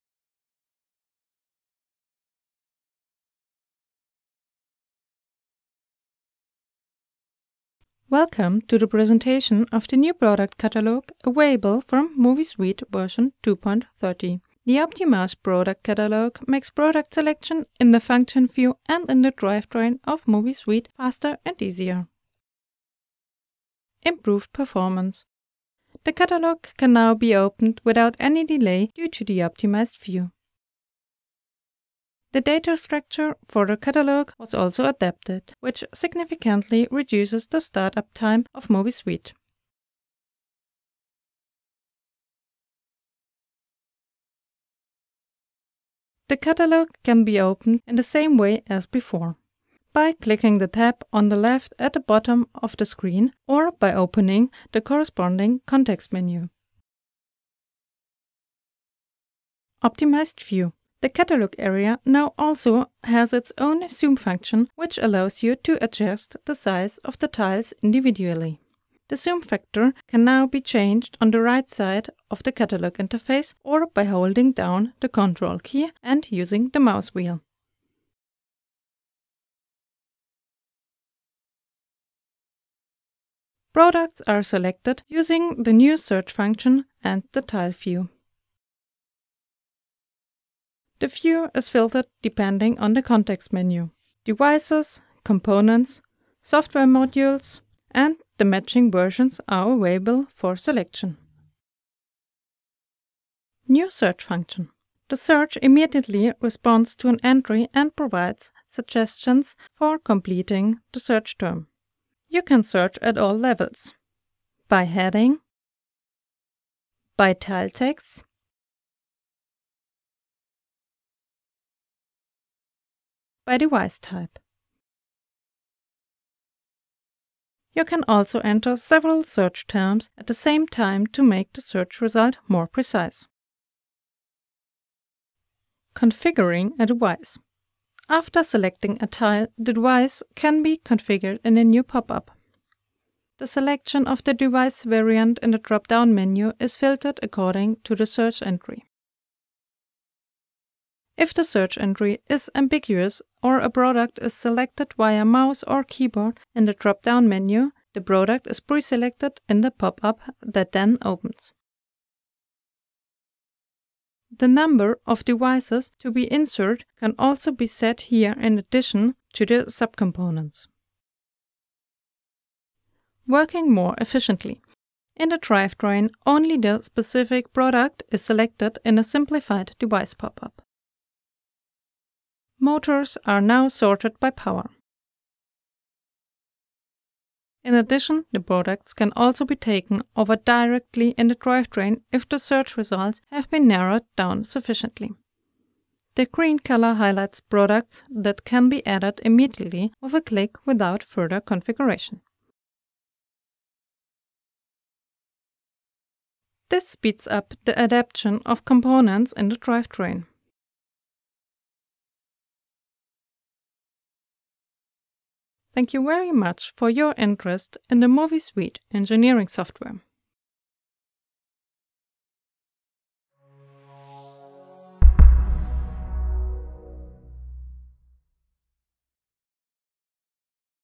New product catalog | MOVISUITE® v2.30 | SEW-EURODRIVE
Welcome to the presentation of the new product catalog in MOVISUITE® v2.30.

New catalog design
- Each device type now has a large icon. After selecting a device type, the required technical data is
requested via a selection menu.
- The search function has been optimized. A search is possible by headings, tile texts and device types.
- The catalog has a zoom function, which allows the size of the tiles to be adjusted individually.